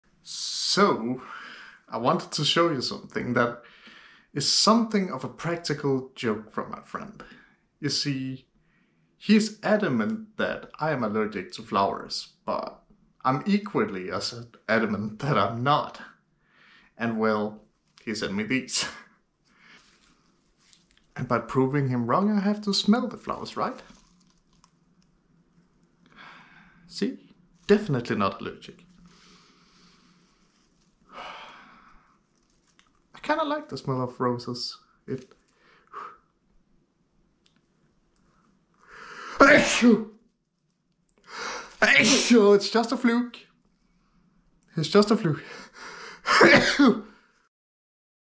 Flower allergy denial 🤧
So, my friend and I have had this friendly rivalry regarding whether I'm allergic to flowers or not. He finally caved and sent me some flowers to "test me".